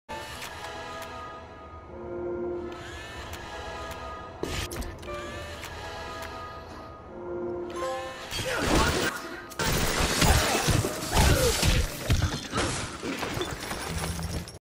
Fully Researched Gun Splicer
Fully Researched Gun Splicer (BRONZE)
The player has fully researched the Leadhead Splicer